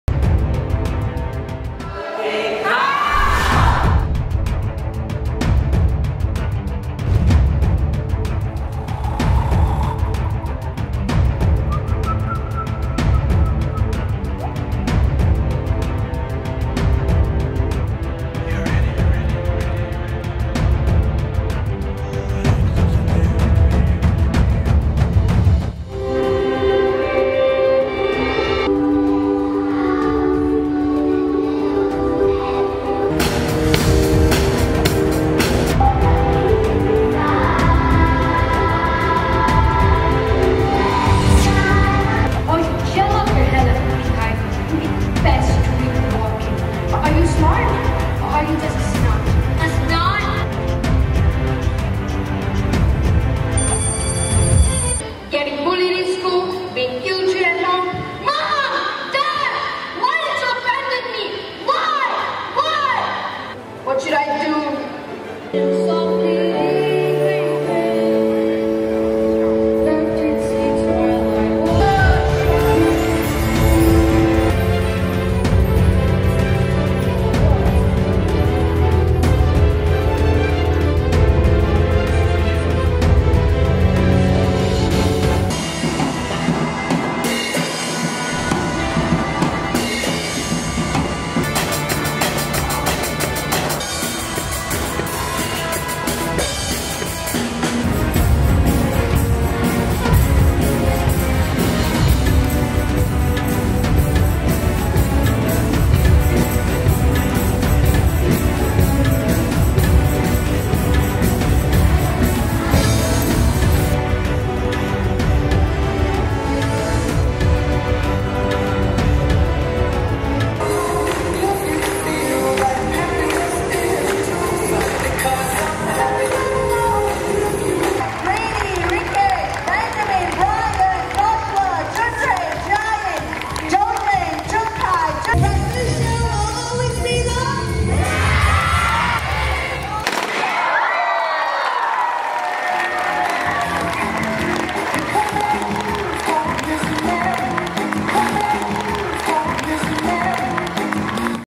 CARL the Musical 2019
Produced by our talented students, Carl is a play about a boy whose life takes a dramatic turn after losing his parents in a car crash. Every aspect of this production, including the storyline, acting, choir, band, props, sound, lighting, wardrobe and more will be done by our students under the guidance of our dedicated teachers. Therefore, this is a wonderful opportunity for them to flex their creativity while gaining knowledge on the entire process.